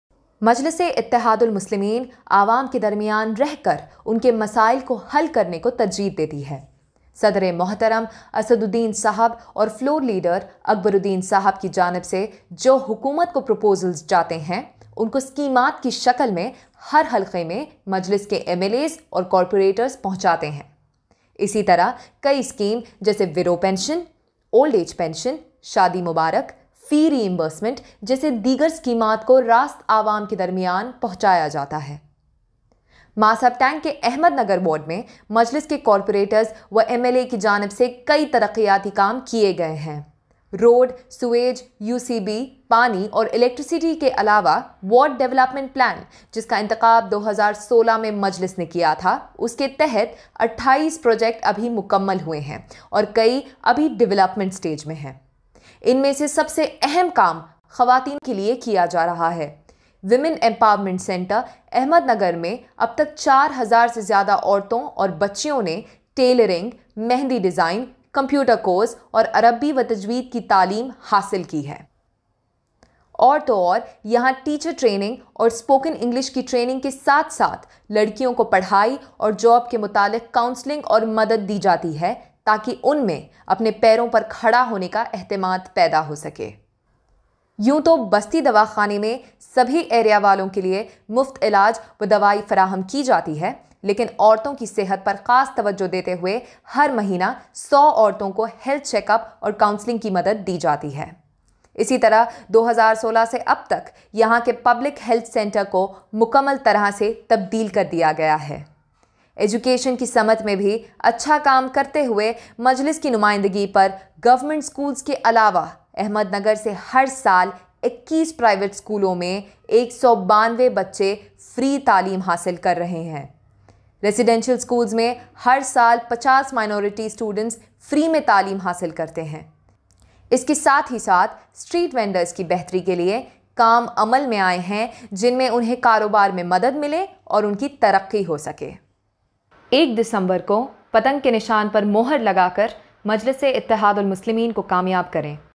Ye Hai Majlis ka Kaam Development Works Done By AIMIM in Ahmednagar Votefor Kite#GHMCelections2020
page : aimim_27

Ye Hai Majlis ka Kaam.

*Development Works Done By AIMIM in Ahmednagar Division...*

Vote for Hyderabad, Vote for Kite